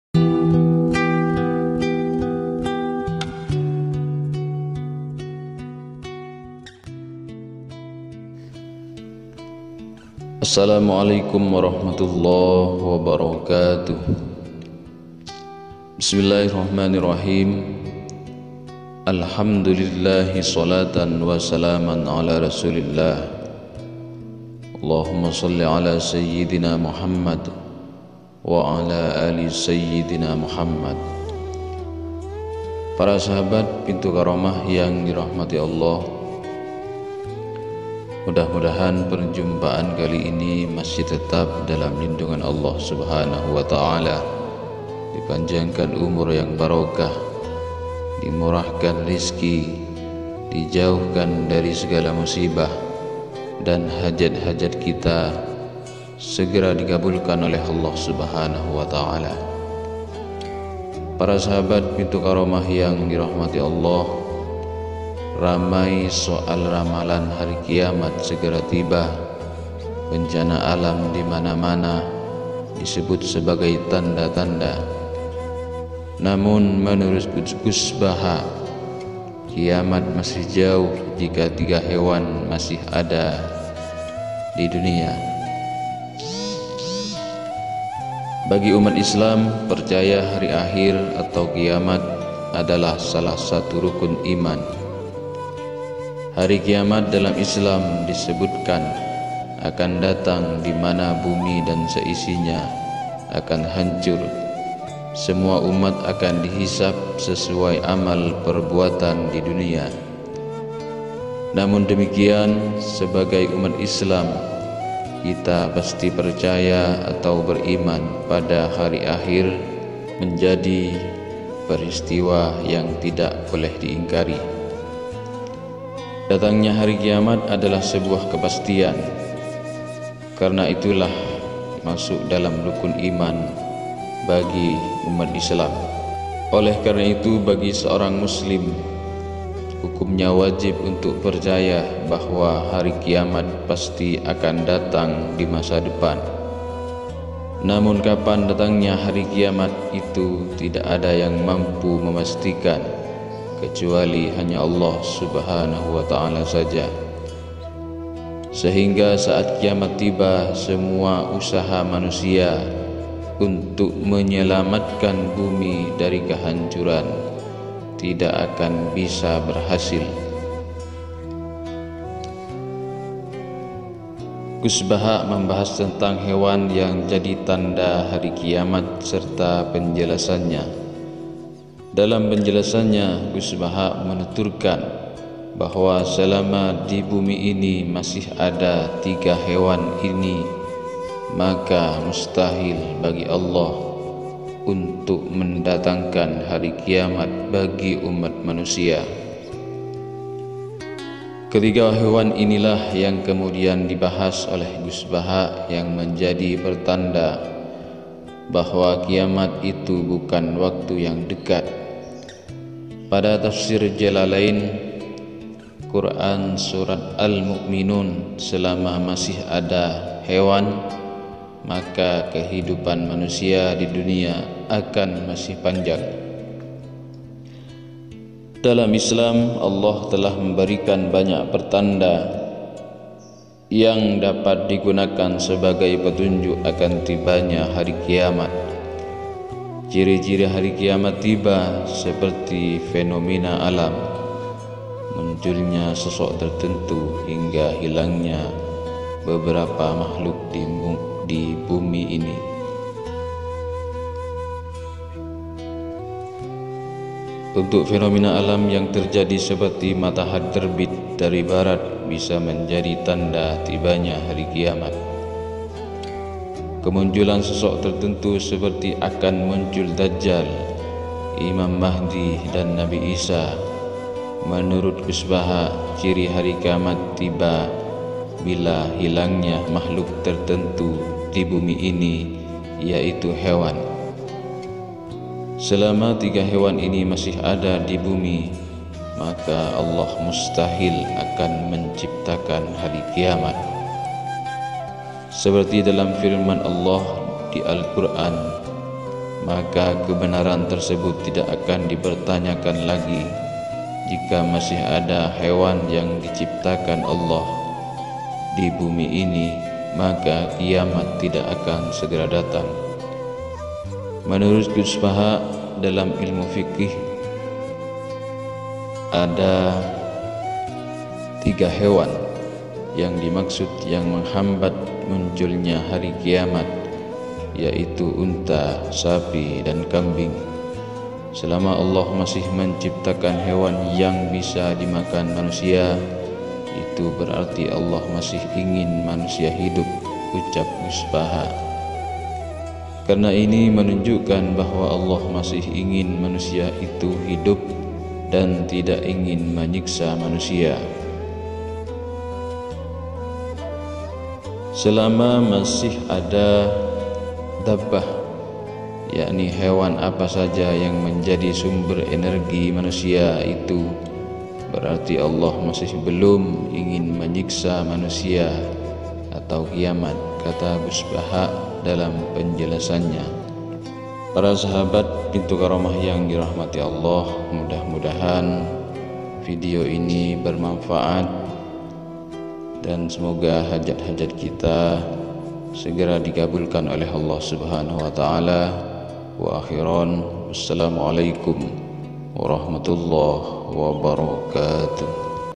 bila masih ada 3 hewan ini didunia, mustahil kiamat akan terjadi || Tanda-tanda kiamat.
bila masih ada 3 hewan ini didunia, mustahil kiamat akan terjadi.

Menurut Gus Baha dalam ilmu fiqih, ada tiga hewan yang dimaksud yang menghambat munculnya hari kiamat yaitu unta, sapi, dan kambing.

"Selama Allah masih menciptakan hewan yang bisa dimakan manusia, itu berarti Allah masih ingin manusia hidup," ucap Gus Baha.

Karena ini menunjukkan bahwa Allah masih ingin manusia itu hidup dan tidak ingin menyiksa manusia.

semoga hajat-hajat kita segera dikabulkan oleh Allah subhanahu wa ta'ala amin amin ya robbal alamin.